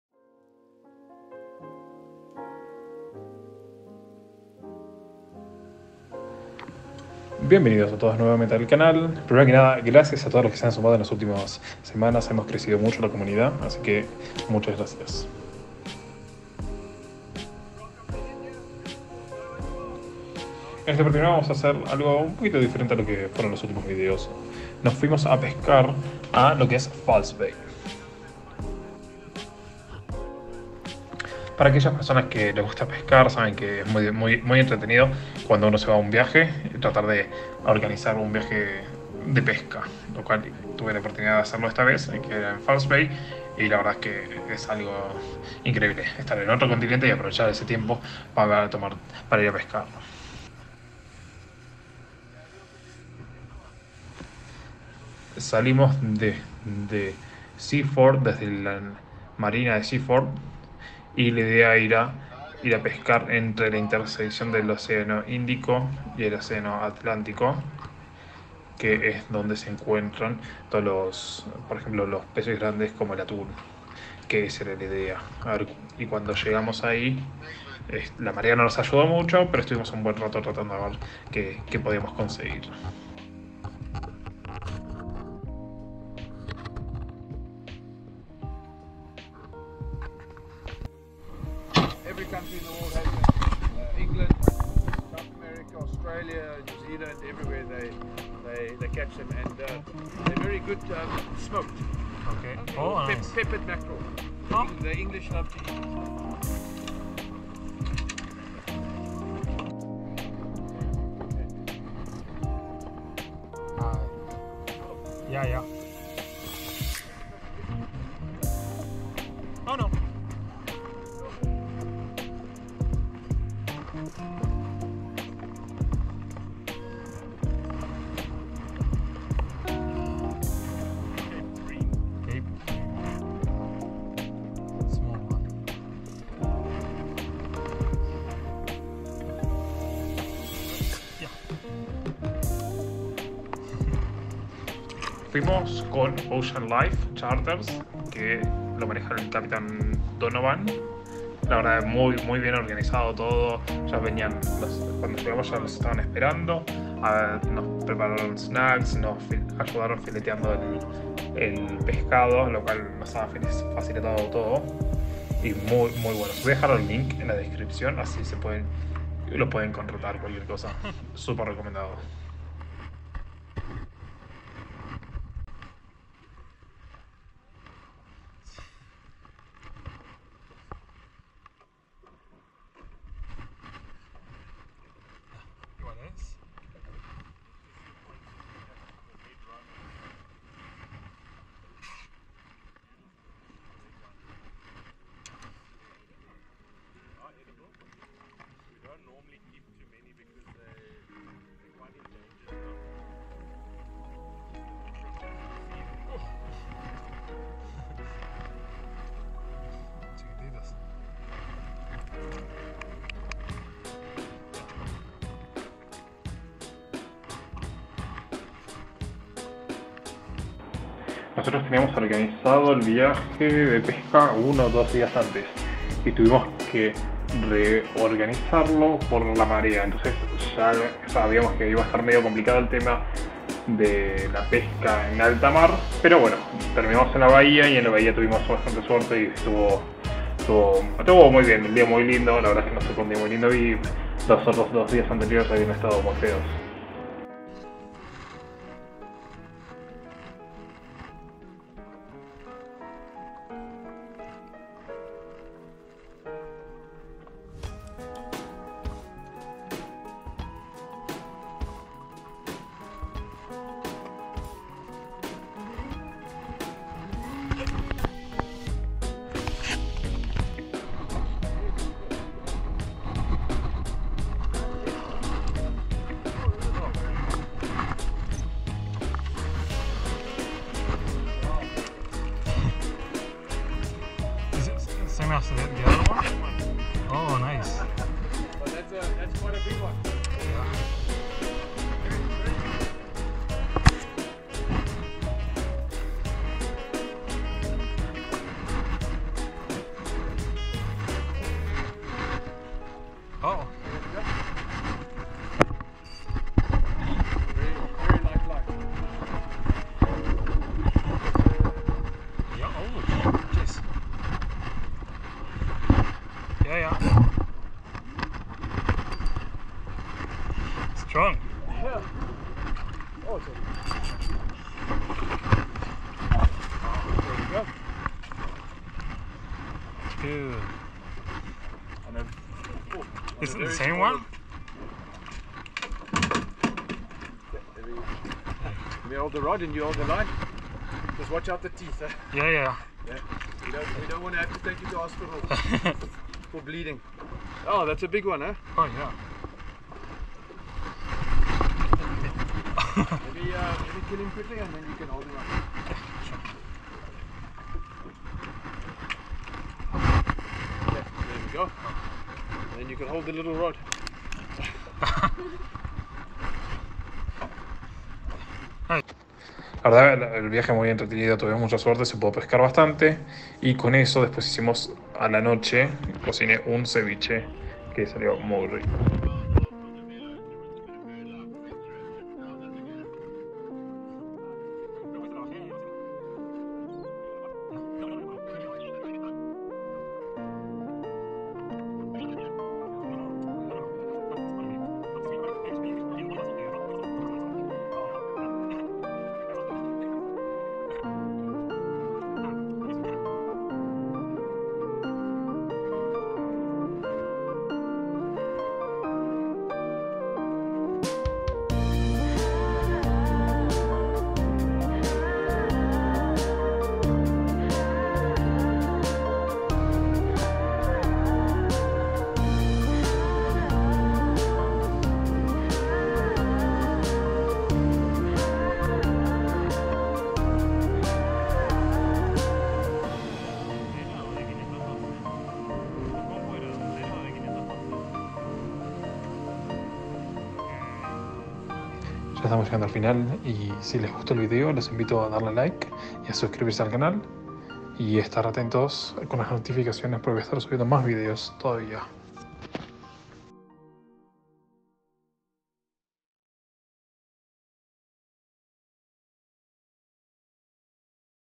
Mañana de Pesca en False Bay | Bahía Falsa | Ciudad del Cabo | Increíbles Peces!
Una mañana diferente en el viaje por Ciudad del Cabo!

En esta oportunidad, fuimos a pescar a mar abierto en la intersección del océano atlántico con el índico!

Lo hicimos con el gran equipo de Ocean Life Charters y su Capitan Donovan!
Una Excelente experiencia la cual se la super recomiendo!